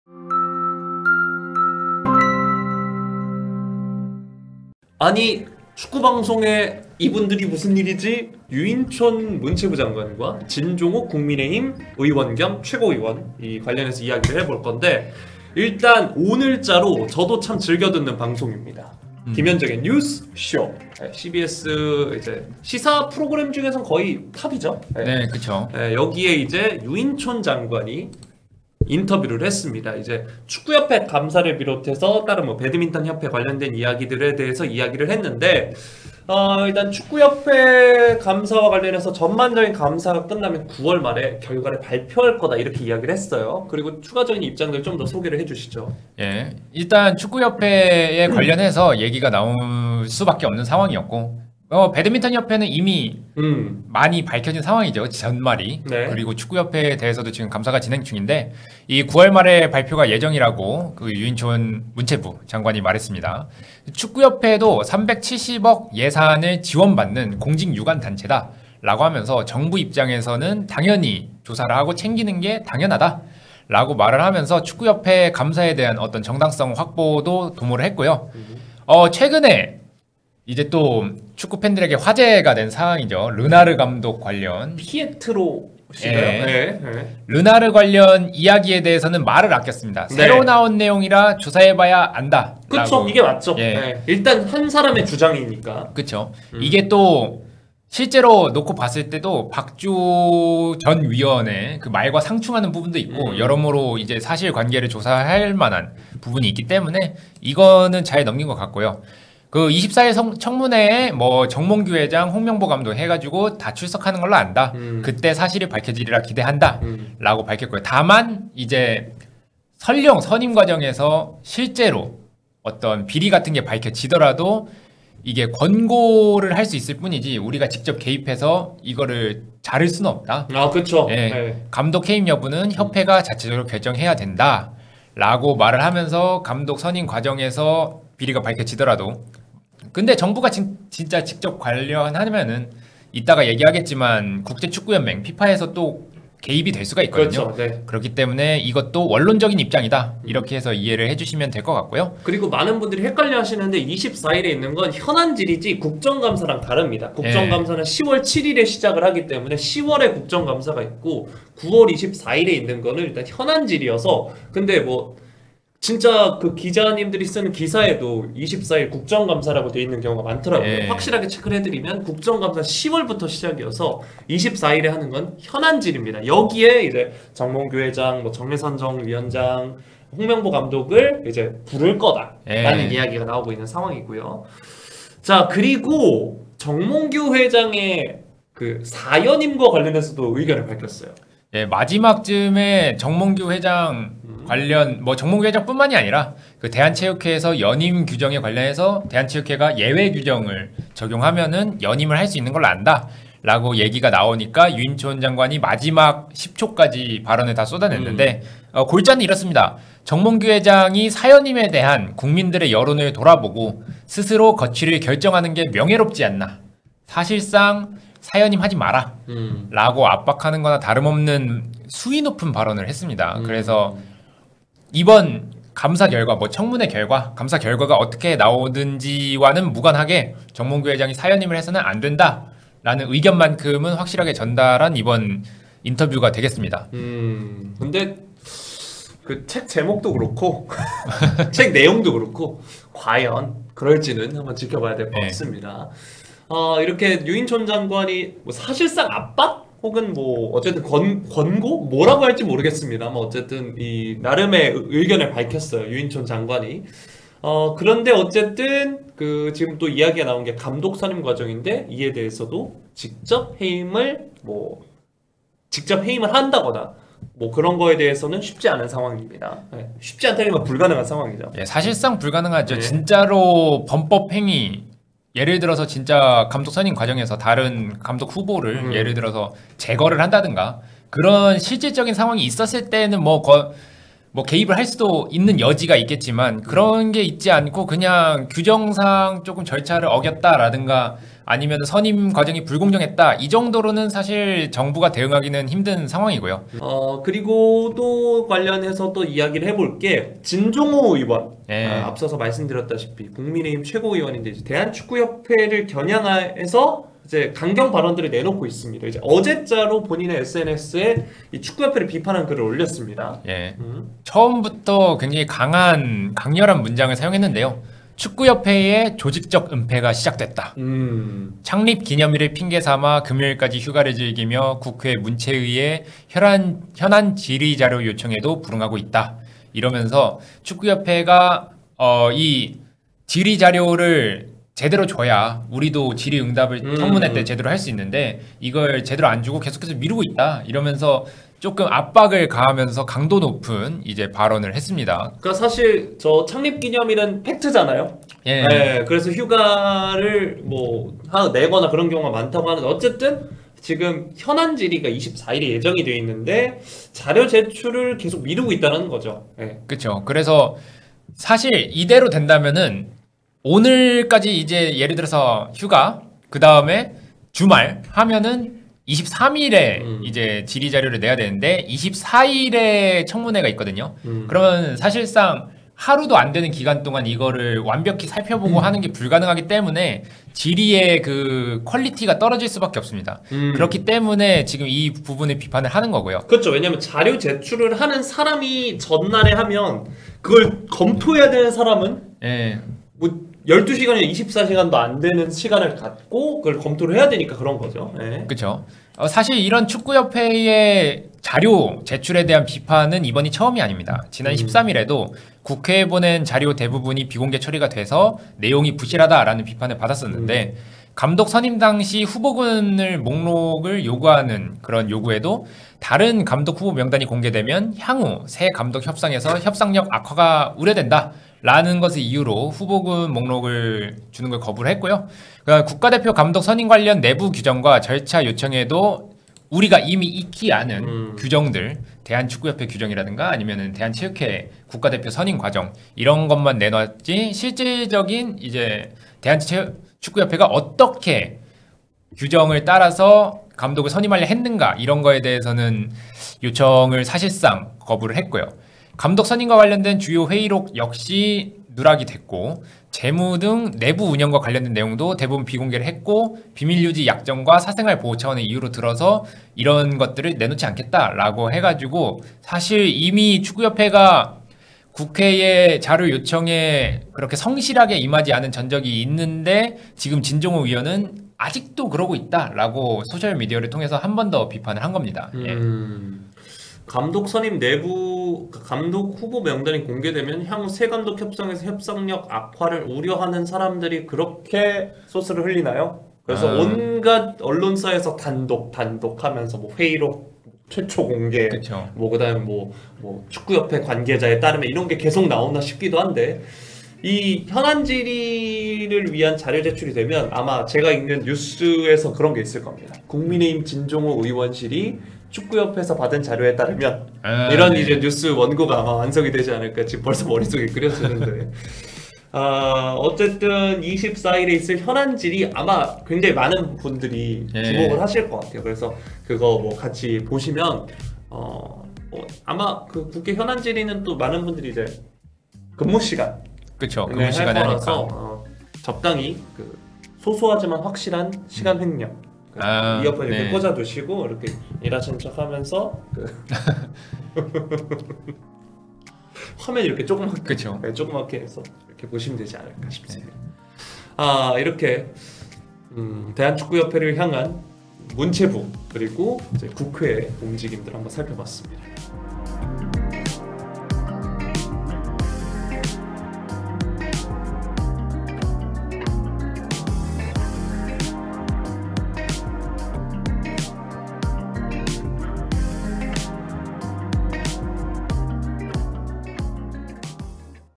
"정몽규 그만둬야…KFA 조직적 은폐" 유인촌과 진종오의 협공
※ 뽈리TV 공식 쇼핑몰